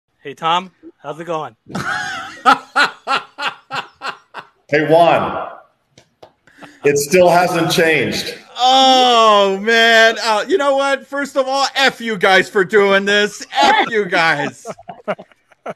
The Bleed Los Podcast - We talk to former Dodgers closer Tom Niedenfuer
On the latest @BleedLosPodcast Therapy was in session for Juan. He conquered his biggest trauma dating back to 1985 and we are all better for it.

FULL EPISODE:
BleedLosPodcast.com

Hosted by Alonso Sarinana, Juan Ramirez and Alysha Del Valle.